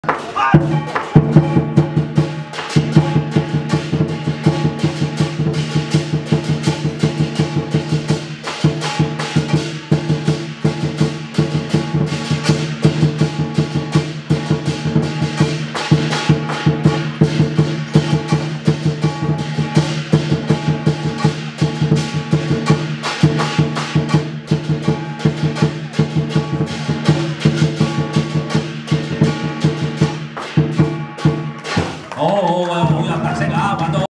John - Bak Mei Hoe 白眉 鋤頭
Bak Mei Vancouver

5 July 2015, Canada Bak Mei Martial Arts Association Banquet